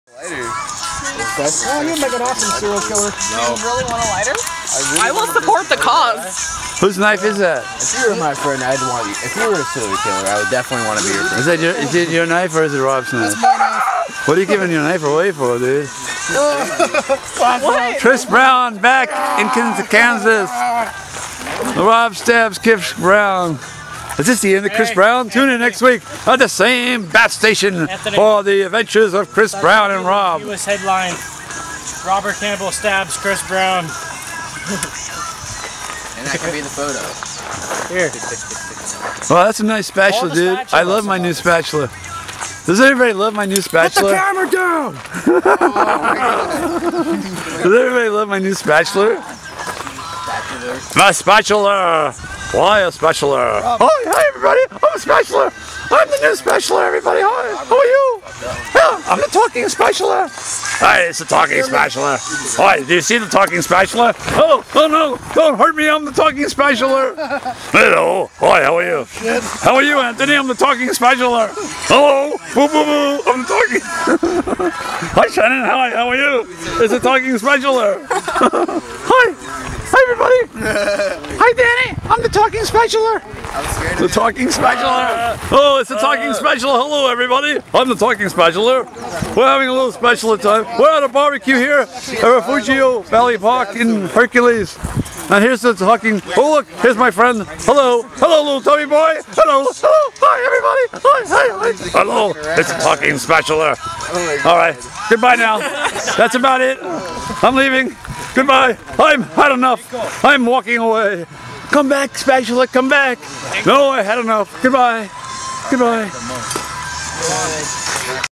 Robert Izs Awesume Meets the Talking Spatula_Refugio Valley Park_April 8, 2014
Robert Izs Awesume Meets the Talking Spatula_Refugio Valley Park_April 8, 2014 with Robert Izs Awesume, Kris Brown, Gabriel Desta, Anthony E. Arndt, Shannon Fontaine, Julie Espinoza, Gabrieal Clark, Daniel Rosillo